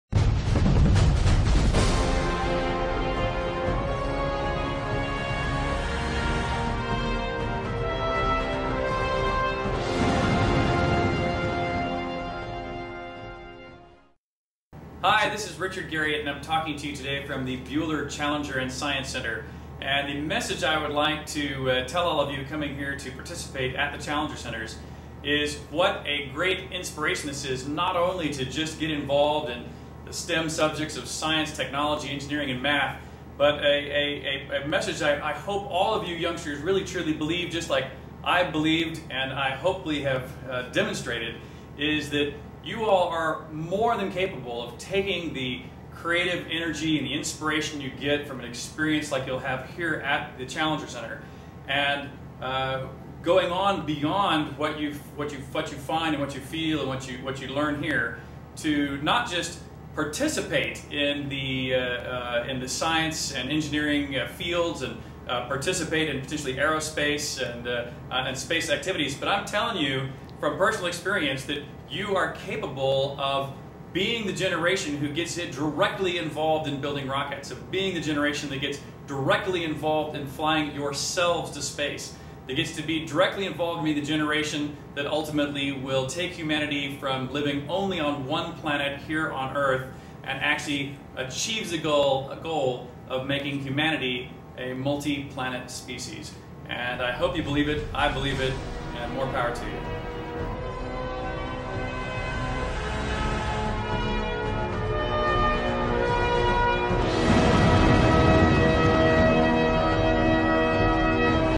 Richard Garriott at the Buehler Challenger Learning Center
Richard Garriott, private space explorer and son of NASA astronaut Dr. Owen Garriott, talks to students about the excitement of a Challenger Learning Center mission and the importance of human space travel and exploration.